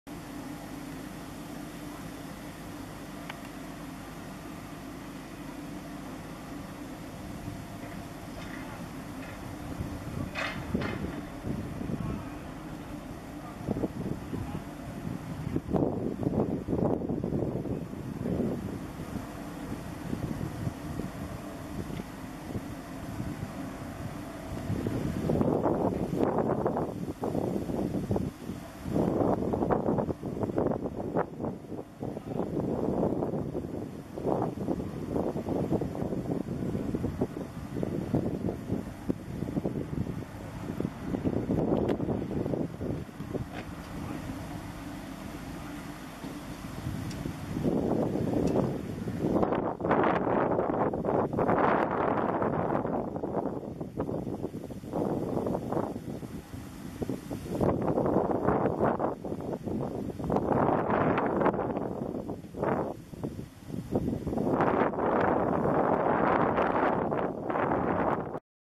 Taucher im Emder Hafen Düker 2 diver in Emden harbor basin
Beim Eindocken des reparaturbedürftigen Schleusentores war auch ein Taucher im Einsatz. Er operierte von der Düker 2 aus.